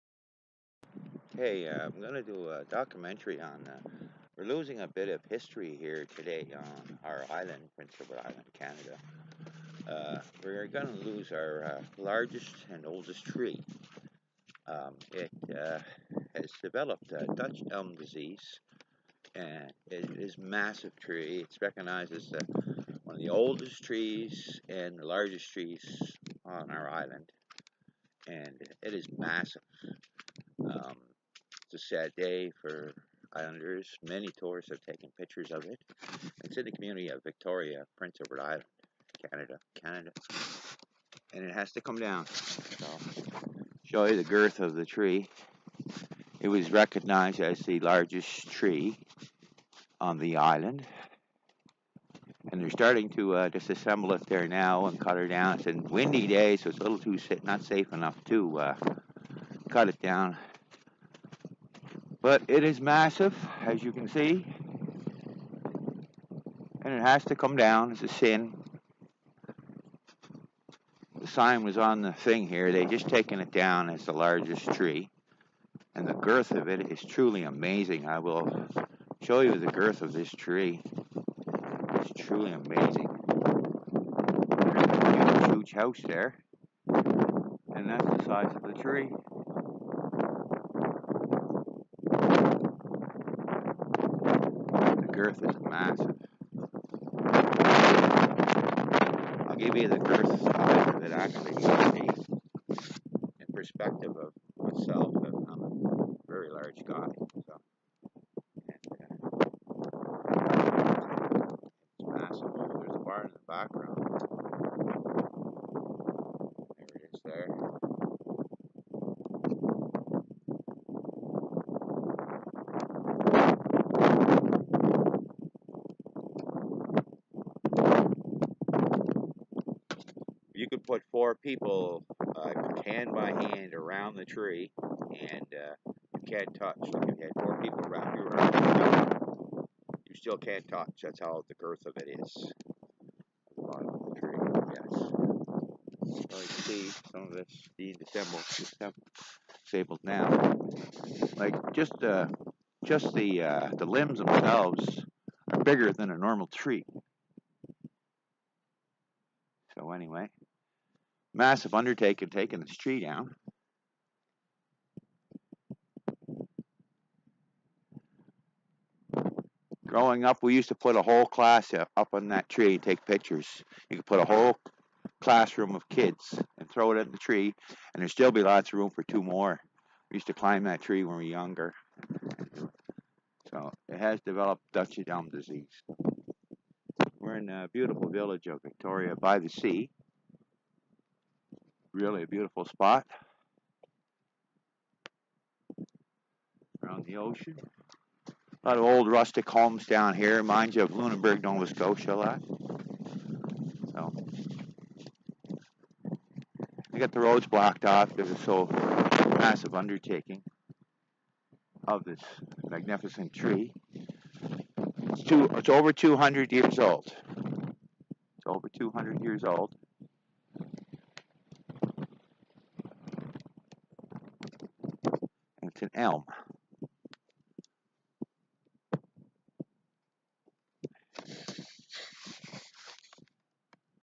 largest tree to come down victoria pei canada
largest tree in prince Edward island to come down 200 year old plus